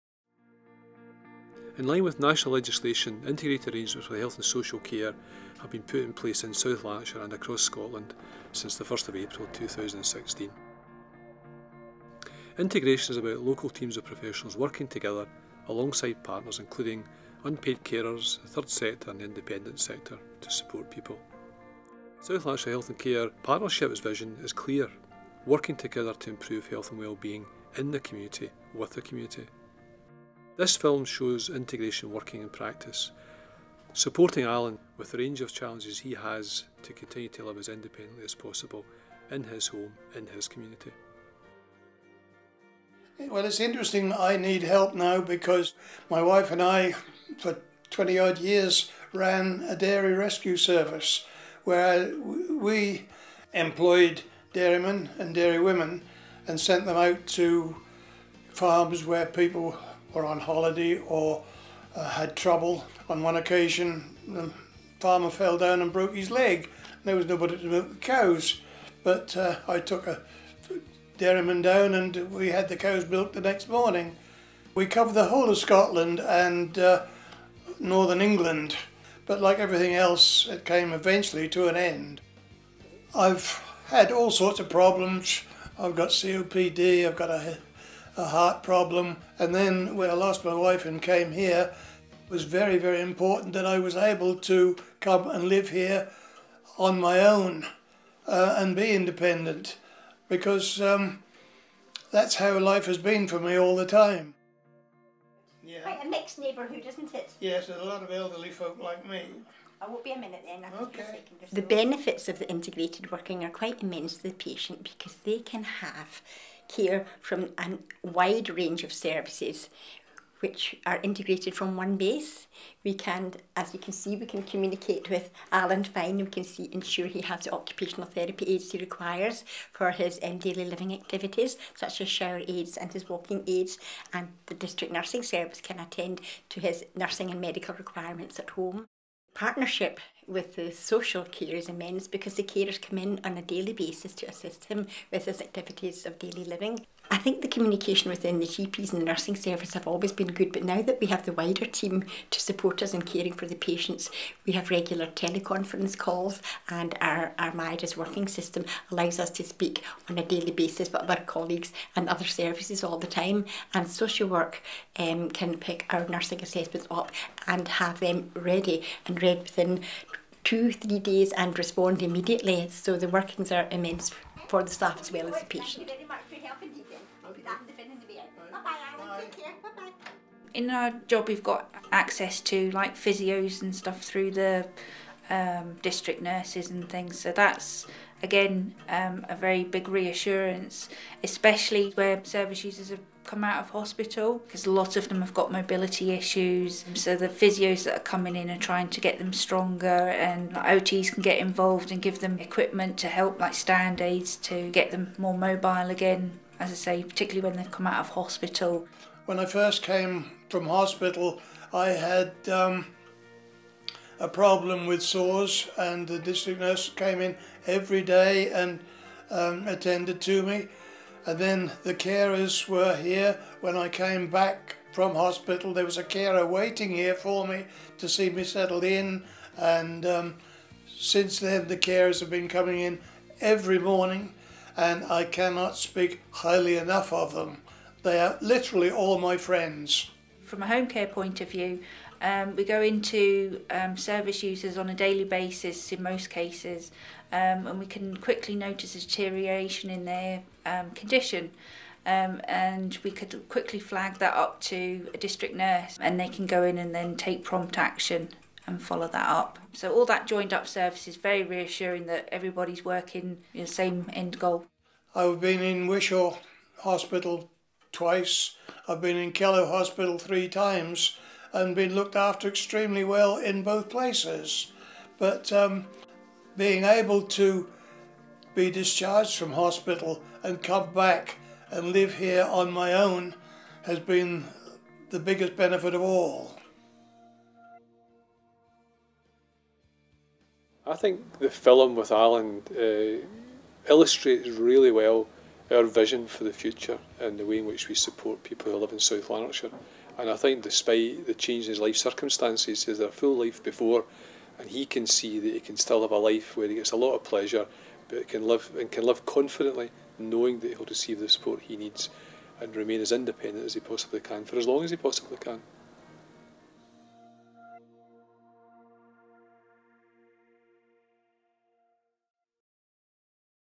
Integration of health and social care in South Lanarkshire
Health and Social Care Integration is underway in Scotland. Allan Snowden from Biggar speaks of his experience of being cared for by South Lanarkshire Health and Social Care Partnership's  Integrated Community Support Team - and how this allows him to continue to live independently in his own home, in his own community.